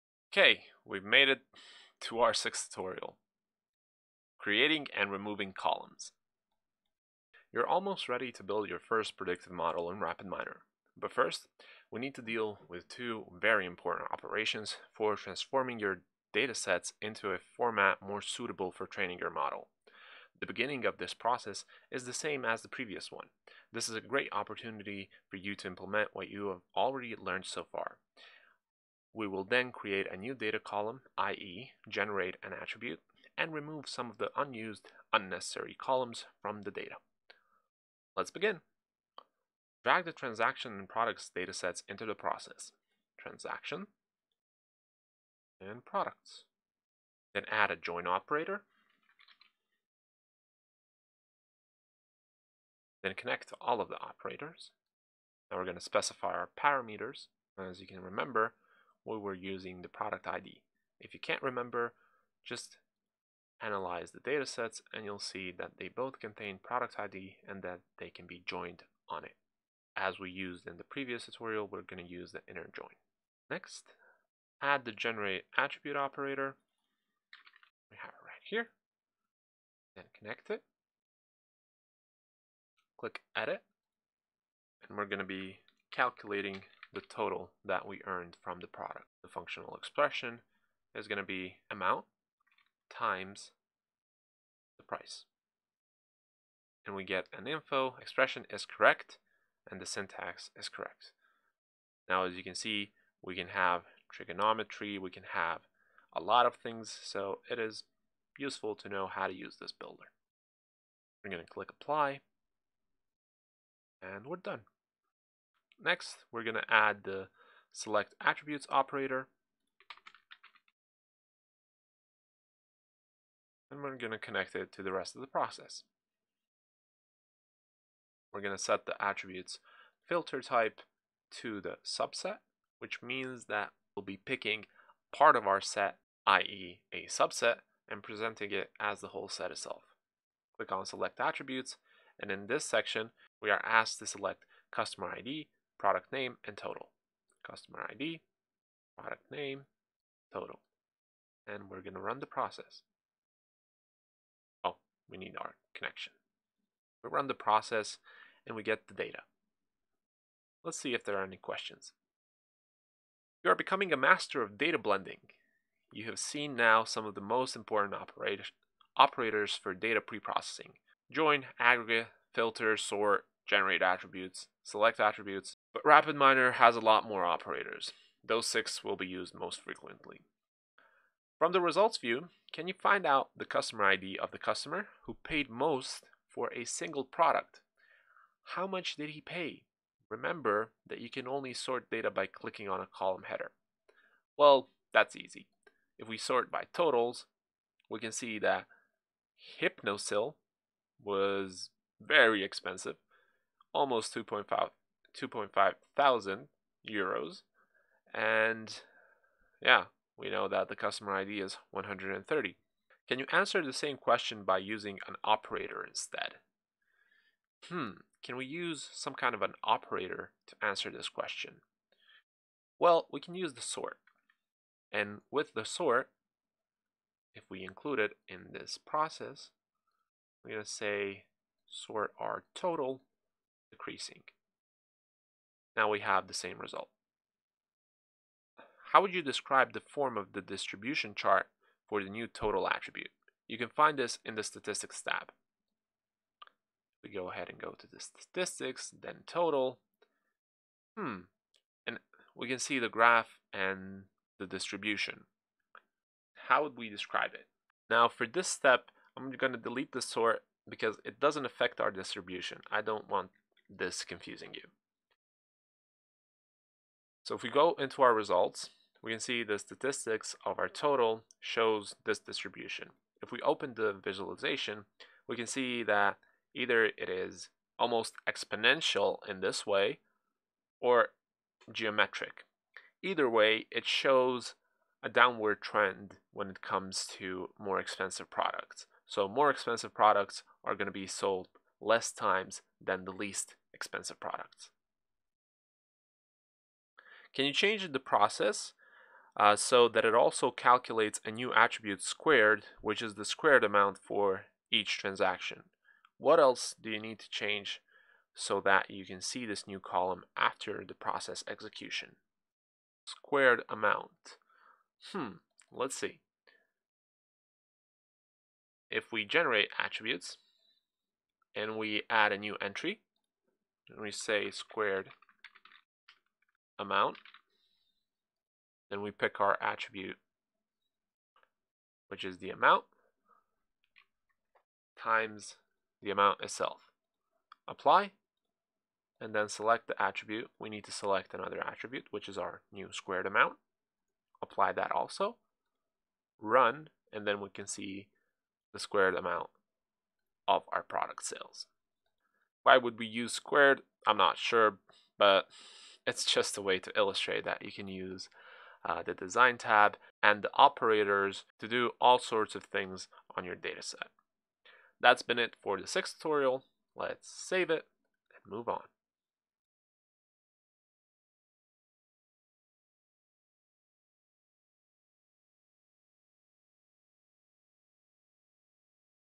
🧠Tut.6 Creating & Removing Columns
Again we join our data, but now we even create some new tributes from our existing data. This computed attribute gives us even more condensed information and summarizes our sales totals. Now we know how to generate the columns we need the most and remove some that aren't of that great of an interest.